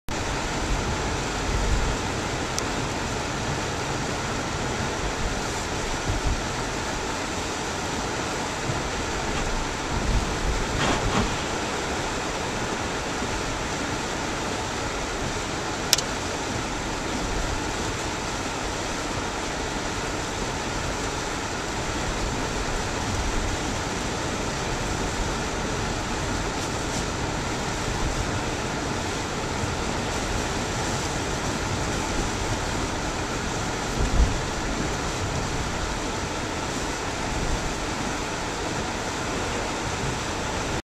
Route 40/322 near Atlantic City, 2/10/2010
Rainy early-morning driving conditions in southern New Jersey on Feb. 10, 2010 ... The rain is supposed to be replaced by snow as the day progresses.